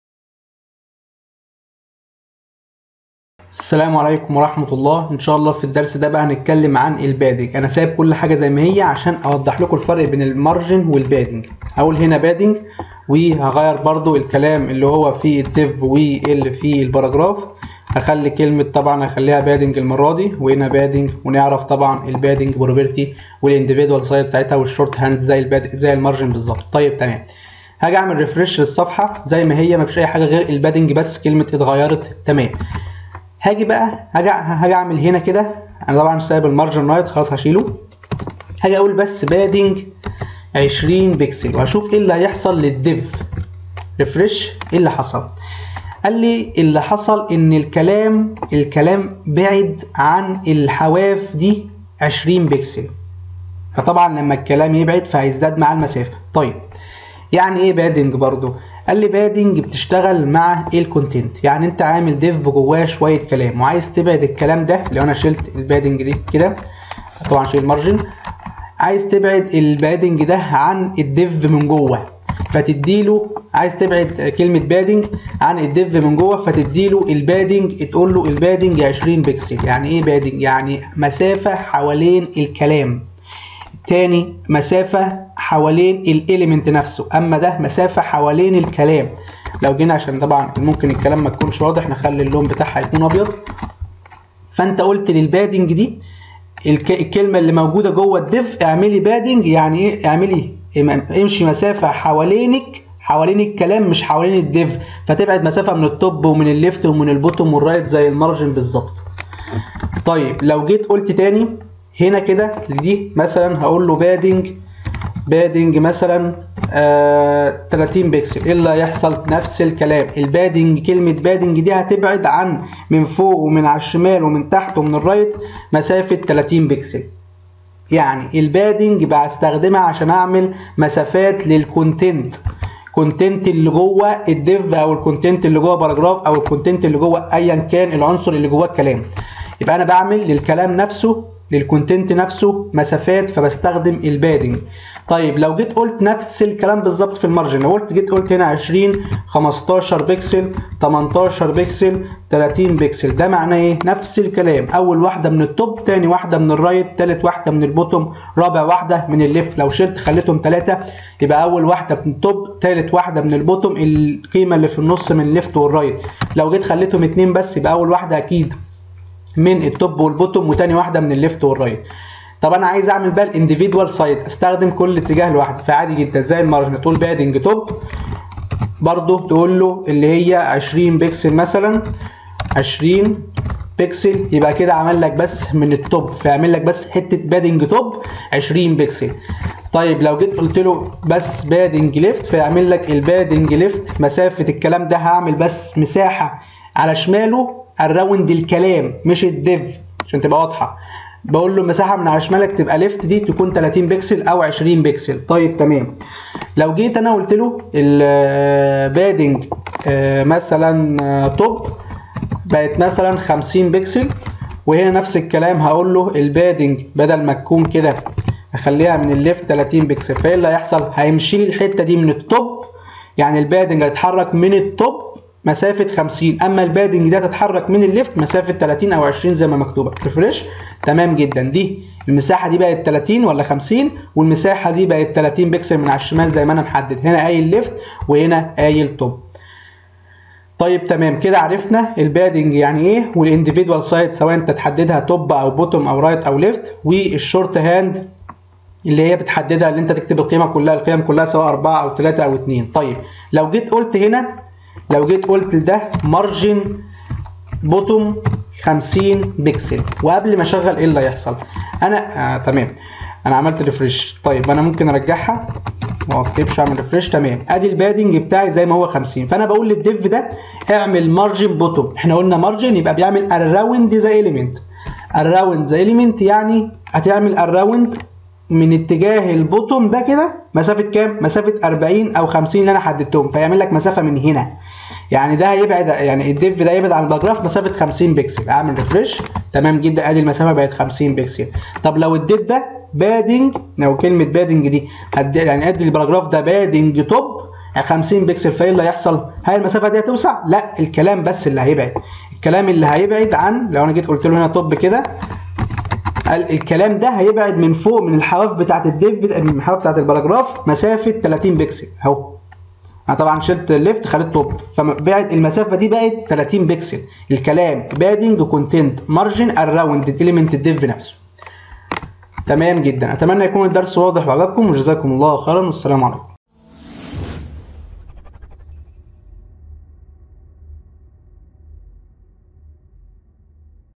8 - ( CSS Tutorial ) Padding Property
شـرح خـاصيـة الهوامش الداخلـية للعنصر Padding ف CSS